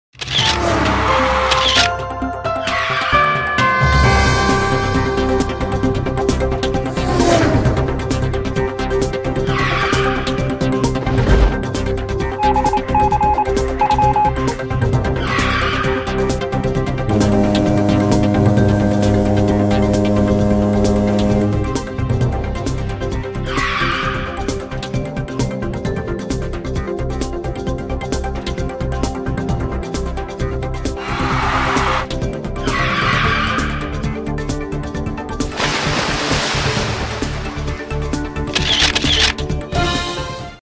Lego race
This is a stop motion of a Lego race. One car hits the other car and it crashes. The paramedics and the firefighters come and save the day!